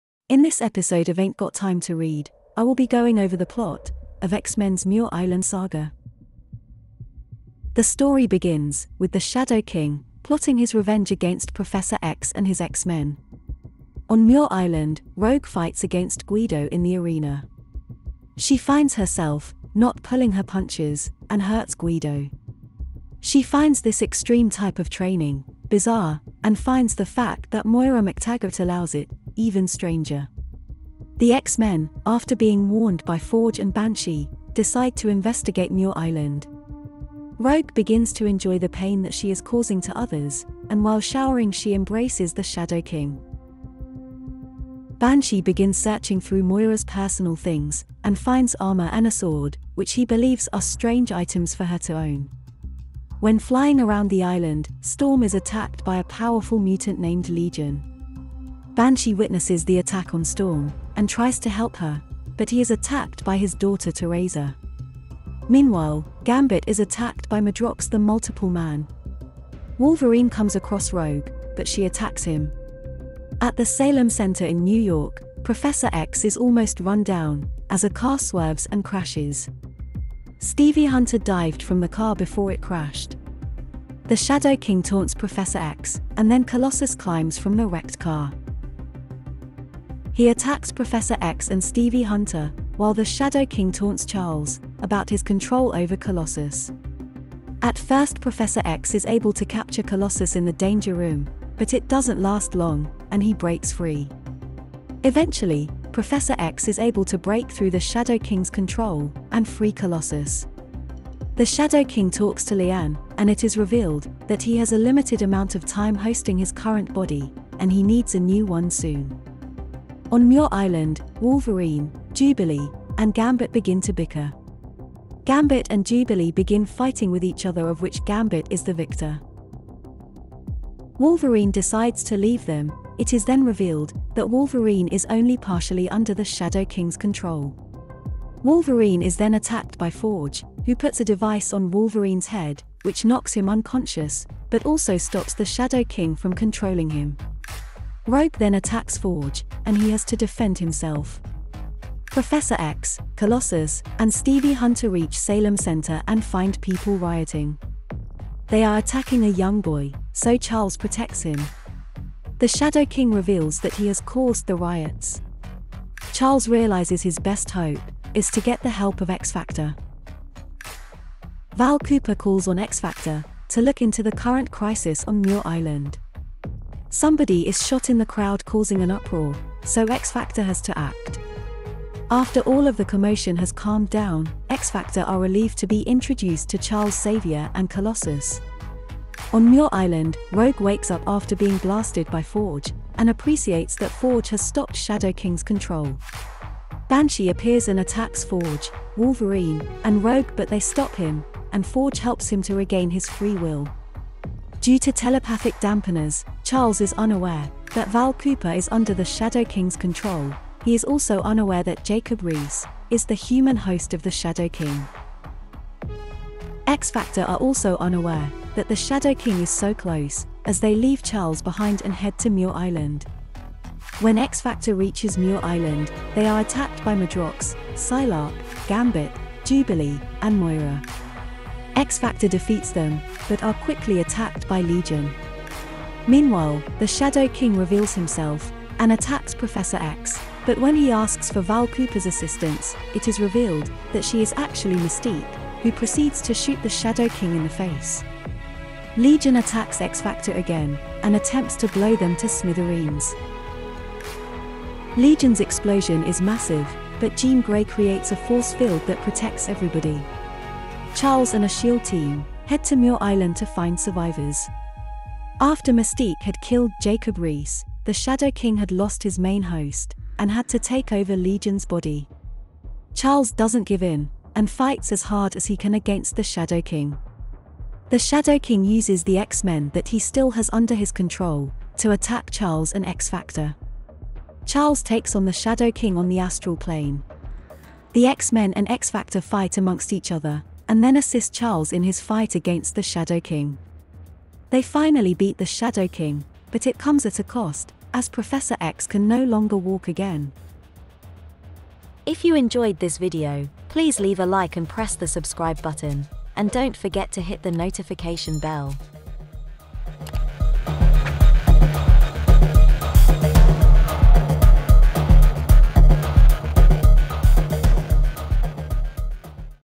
Uncanny X-Men Muir Island Explained | X-Men Crossover
Watch our video Uncanny X-Men Muir Island Explained | X-Men Crossover | X-Men Explained today to see our comic book plot summary of the X-Men Muir Island storyline.

X-Men Explained - The Muir Island Saga is a five-part Marvel Comics crossover event involving the X-Men and X-Factor, comic books published in 1991. It was written by Chris Claremont and Fabian Nicieza.

In this X-Men and X-Factor crossover Professor X, Moira McTaggart, Forge, Banshee, Storm, Legion, Madrox The Multiple Man, Colossus, Wolverine, Gambit, Jean Grey, Beast, Iceman, Angel, Psylocke, Mystique, Jubilee, and Cyclops work together to defeat the Shadow King in the Muir Island Saga.

Watch our video explaining The Uncanny X-Men comic book storyline, The Muir Island Saga.

Purchase X-Men Muir Island At Amazon

SPOILER ALERT - I would always advise that you read the story yourself but these videos are aimed at those that do not have the time, which means that plot spoilers are present within the video.